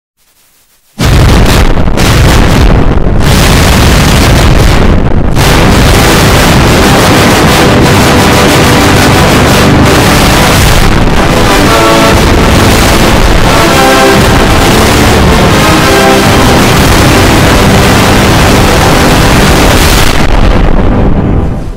(WARNING: LOUD) 1996 20th century fox home entertainment has a seizure virus
Video: X major 777 in X is Vomiting in G major 1 in Right mirrored

Audio: X is Vomiting in does respond in mangle effect